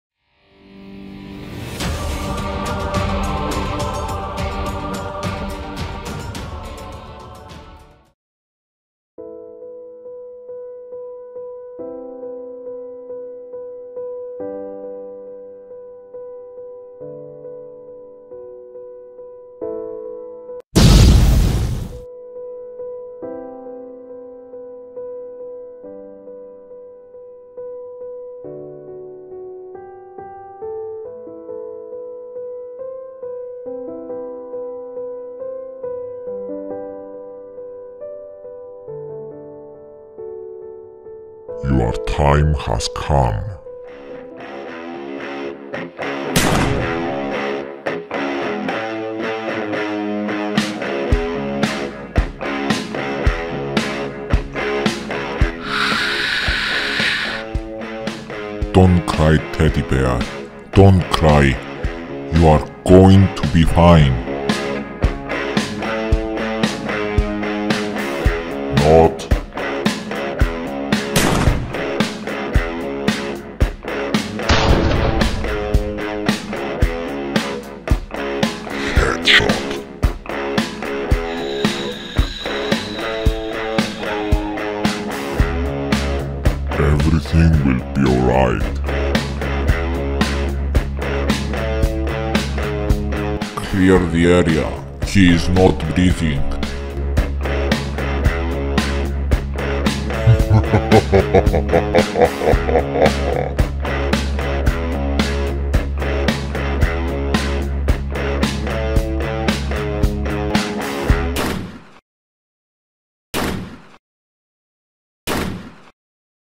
TEDDY BEAR SURVIVES HELICOPTER CRASH?! - Part 2
This poor Teddy Bear was  a survivor from a helicopter crash but then the Hunter shoot and fatally wounded him.  The Teddy Bear is in a critical condition now.